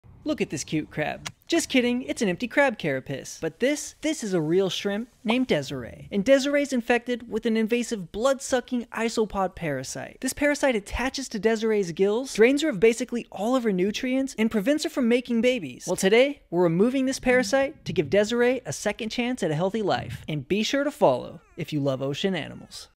Removing Blood Sucking Parasites from Desiree￼
A crab care of his is a Crabs Shell that has been shed off of the crab. This happens when crabs get too big for their shell. Desiree got her Parasite removed And now she’s feeling much better!